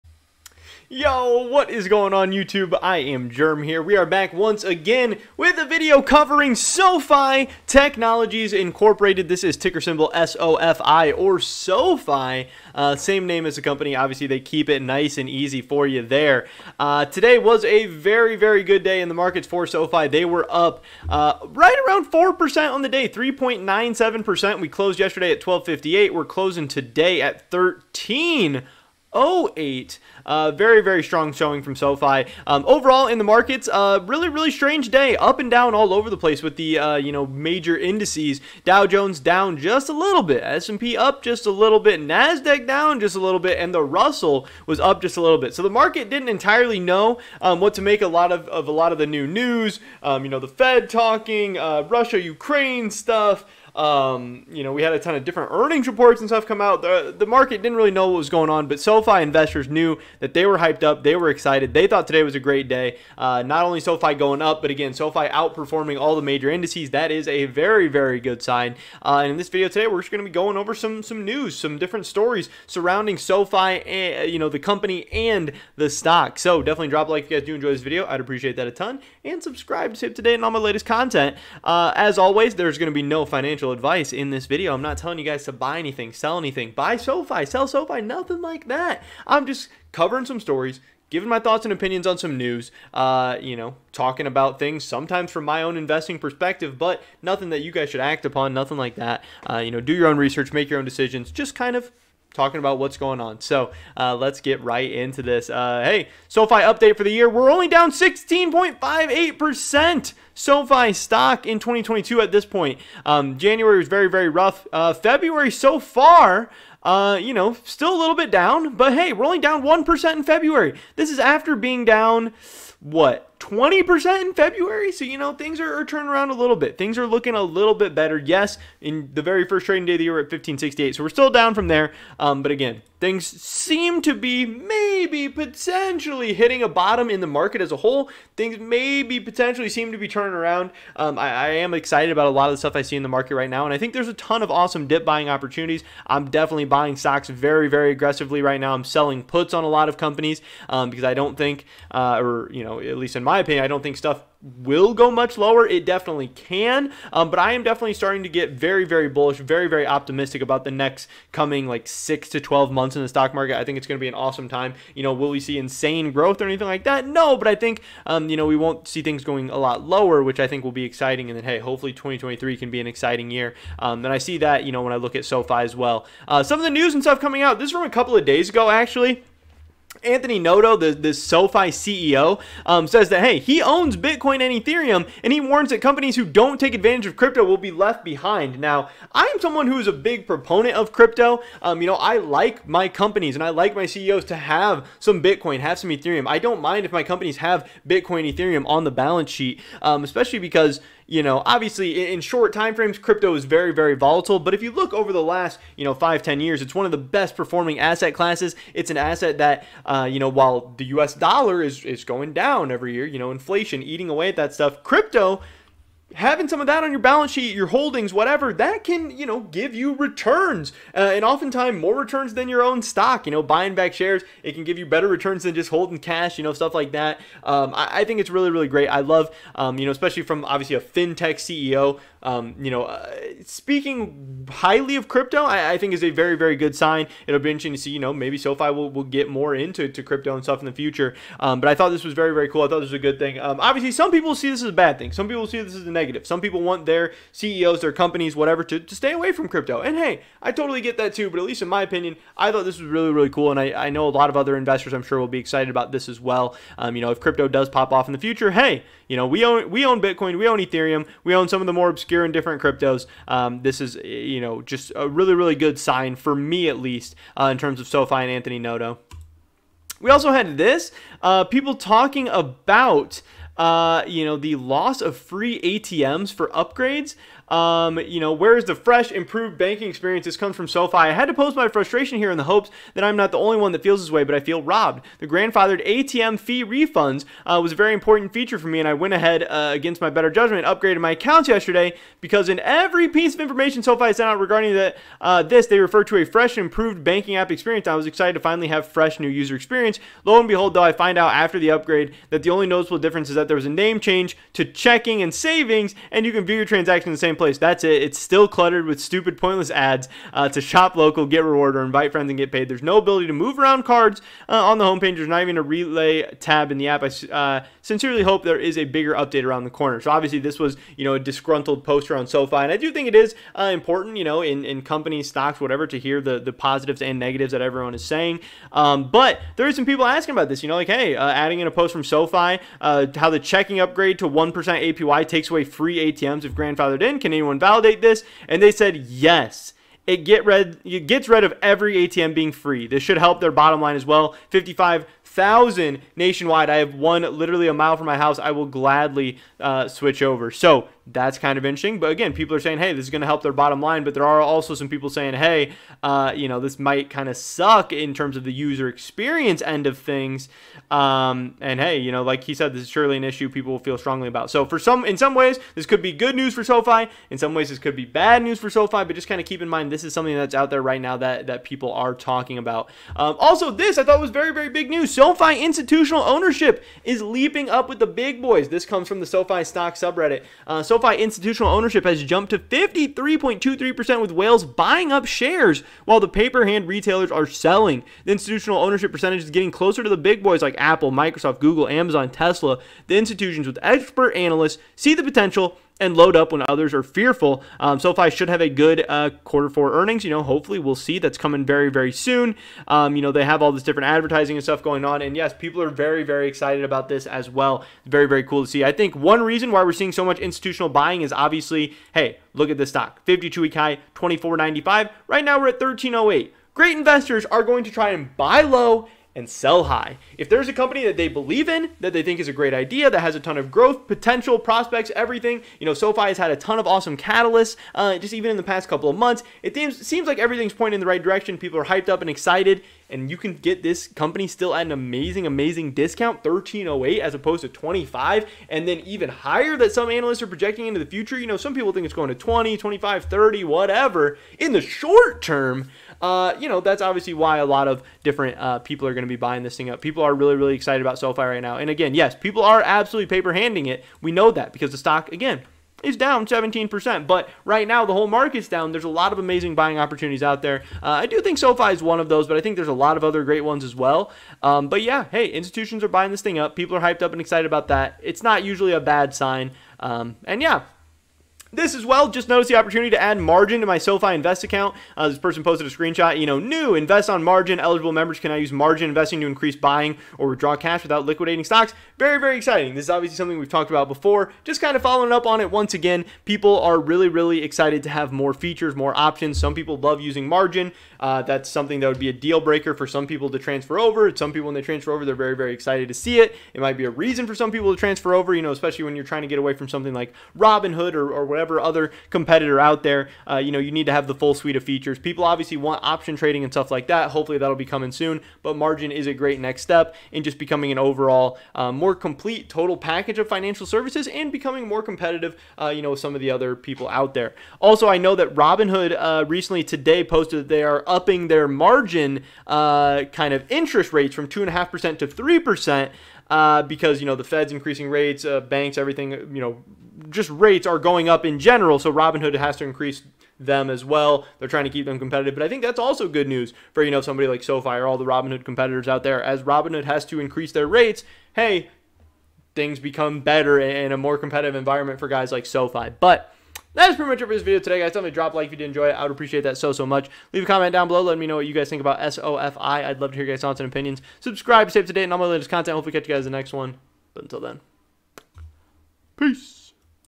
BIG SOFI STOCK NEWS - "WE OWN BTC ETH AND OTHER CRYPTO" | MORE MARGIN TRADING | INSTUTIONAL MONEY
I hope you enjoy and your feedback is always appreciated :D